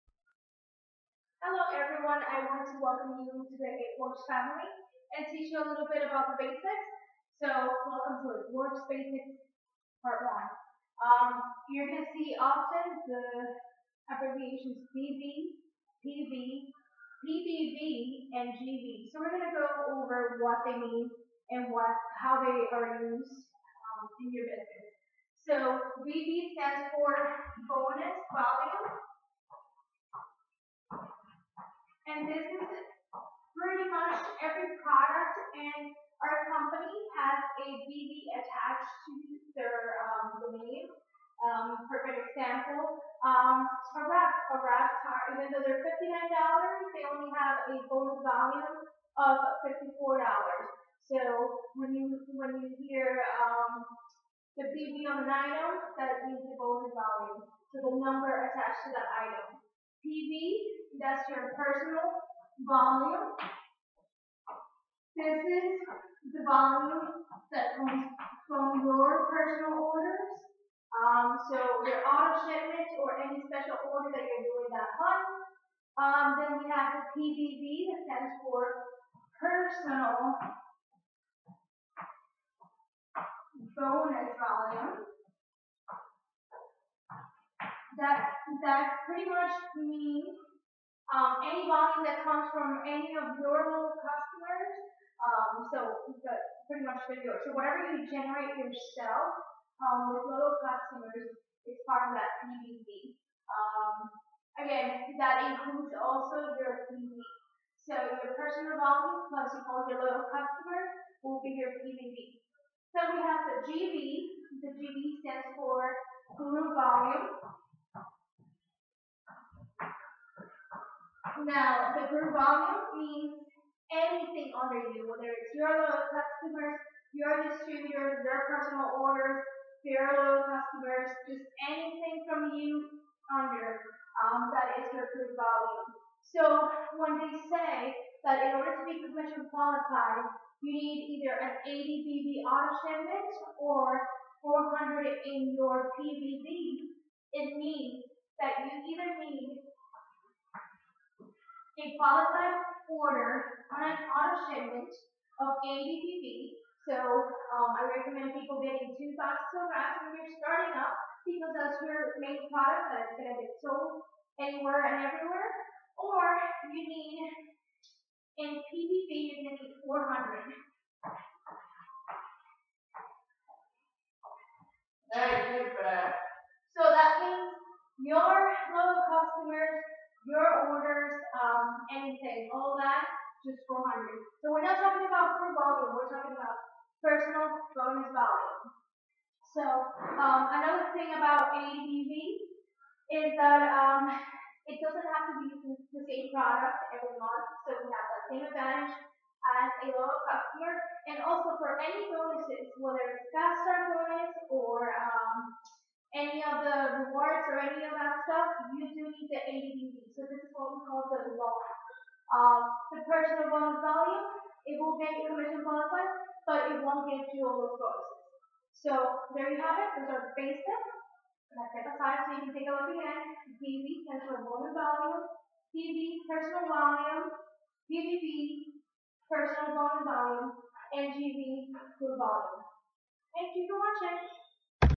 It Works Distributor Basic Training Pt. 1
Did you just sign up as a DT ( Distributor) and don't know what BV, PV, PBV or GV means?